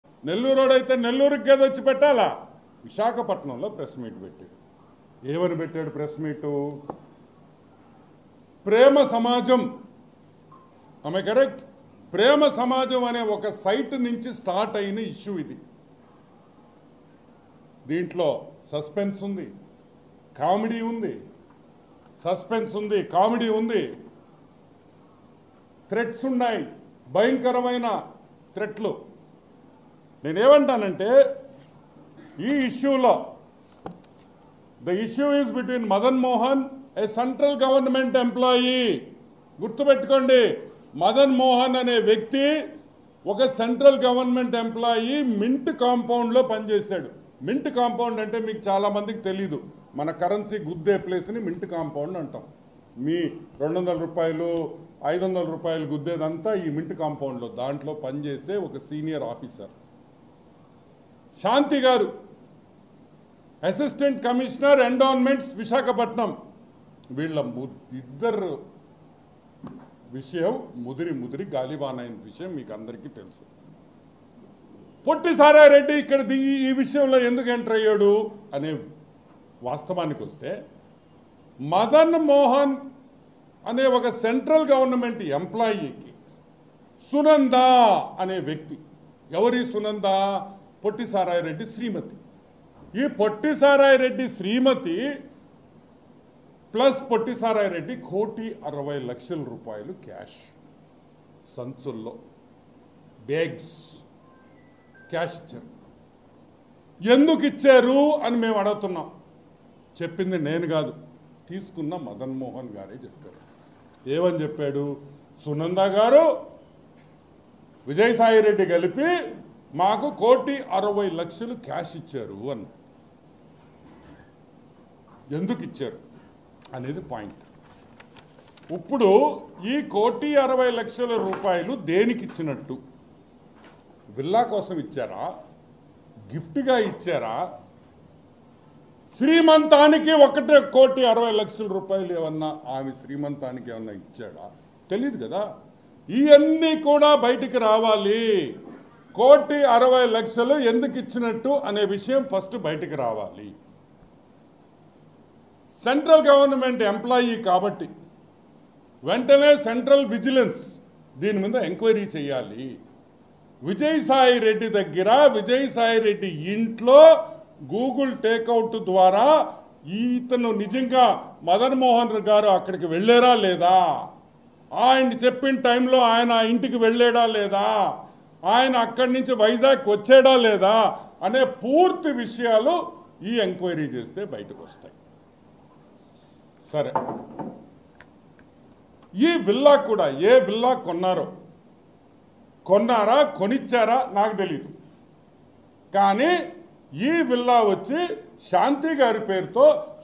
పొట్టి సారాయి రెడ్డి ఆ కోటి రూపాయలు ఎందుకు ఇచ్చావ్..? | Anam Over Mandan Mohan Comments | ABN Telugu
🔴LIVE : TDP Anam Venkata Ramana Reddy Sensational Press Meet || ABN  Telugu